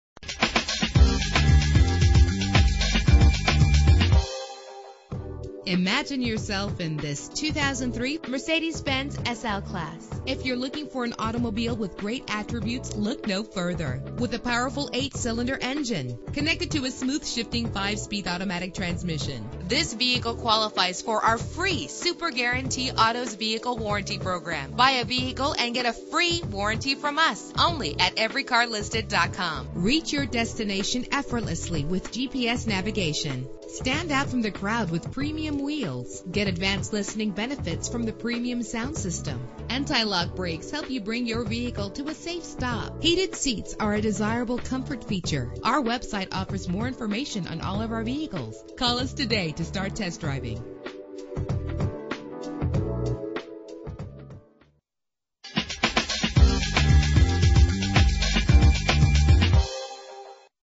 2003 Mercedes-Benz SL-Class - Sarasota FL
Year: 2003
Make: Mercedes-Benz
Model: SL-Class
Trim: SL55 AMG
Engine: 5.4 liter 8 cylinder 24 valve
Transmission: 5-Speed Automatic
Color: Brilliant Silver Metallic
Mileag